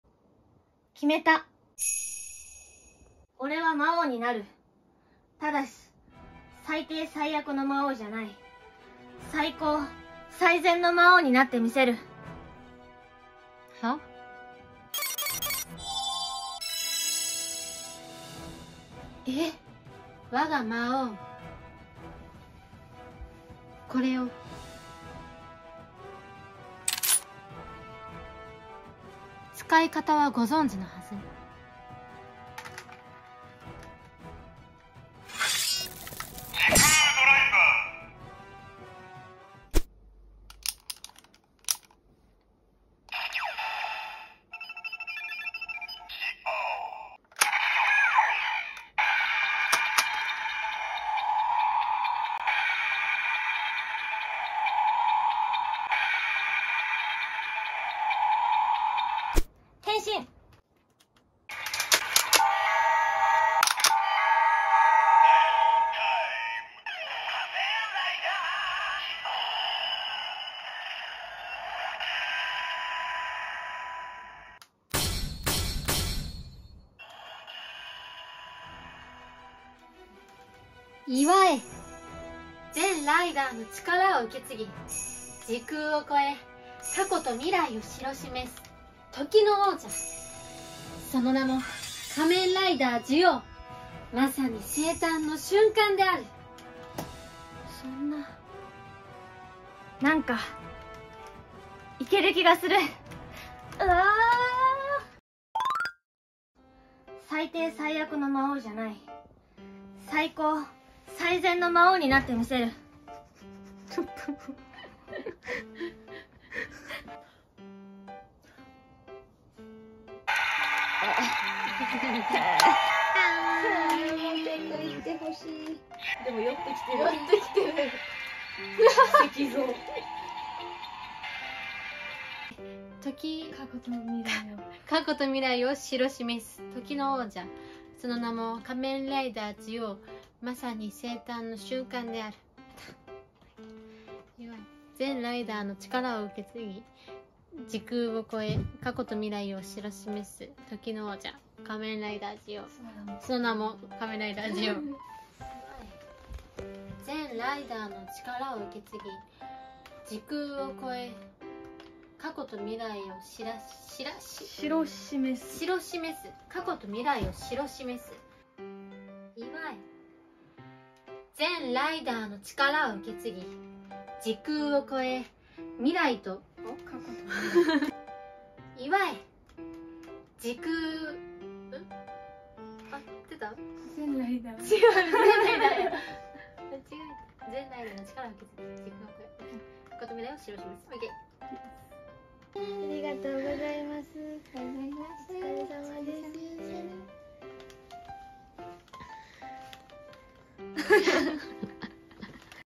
【変身再現】仮面ライダージオウの初変身を再現してみた！
今回はジオウの初変身をしてみました！
ライドウォッチのカチカチ音が大好きでいつも回してました❤️‍🔥

【出演】